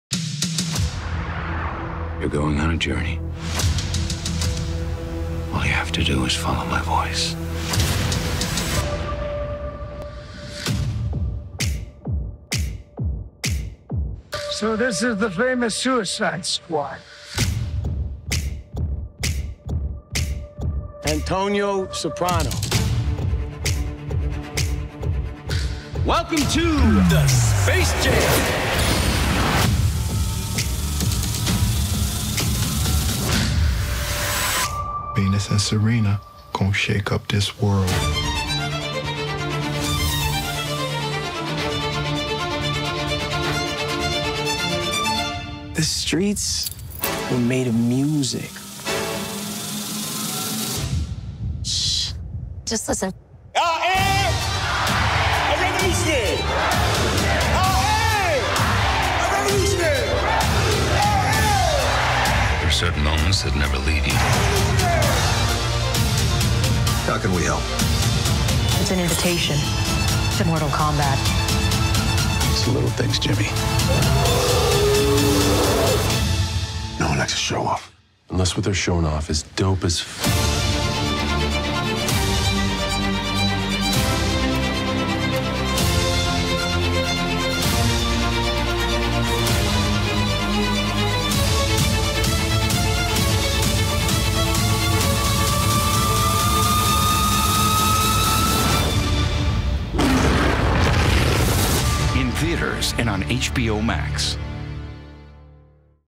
Same Day Premieres "Trailer"
This year every Warner Bros. Pictures movie will be in theaters and streaming exclusively on HBO Max.